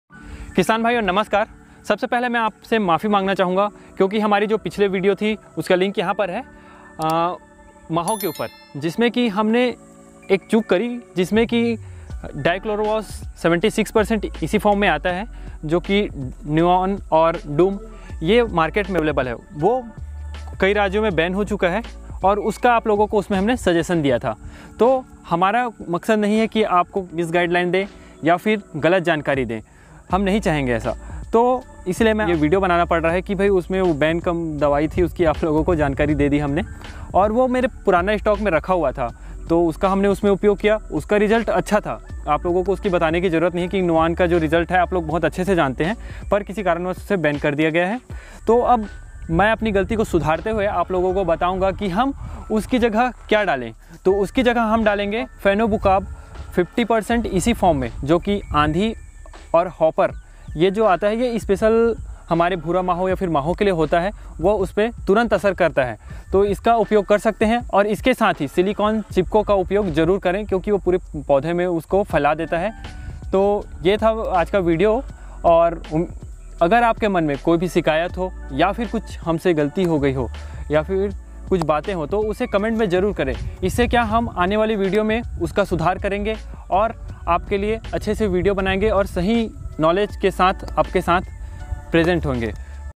Nuvan Insecticide Ban । Dichlorvos 76% EC BAN । Sorry kishan bhai 🙏
किसान भाई हमने एक वीडियो में नुवान जो कि बंद हो चुका है लेकिन हमारे पास था । वीडियो बनते समय इसकी जानकारी नहीं थी। इस लिए हम आप लोगो से माफी मांगते हुए। उसकी दूसरी दवाई बताएंगे।

Nuvan Insecticide ban ho gya kya
dichlorvos 76% ban ho gya kya
kya boom ban ho gya

धान का भूरा माहू। Brown plant Hopper.dupont pexalone,syngenta chess